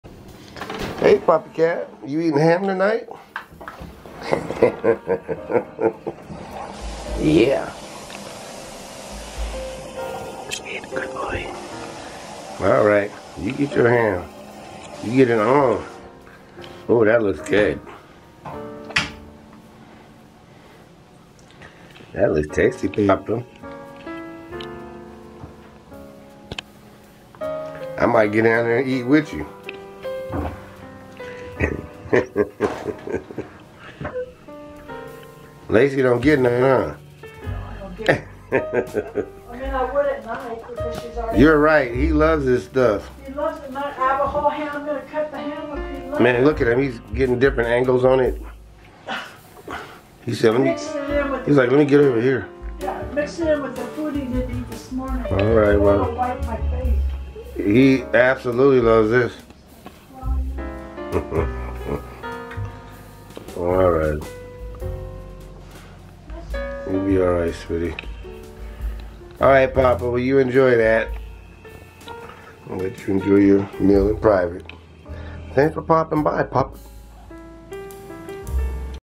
Papa Cat Eats High on the Hog
It's a ham dinner for ole Papa Cat.  He LOVES it.  He has to eat fast before Lacey Cat shows up.  He's really wolfing it down though, so no worry.  I'll feed Lacey Girl something else.

I hope you are eating your fill tonight, and are satisfied.

Please visit this wonderful cat

PEACE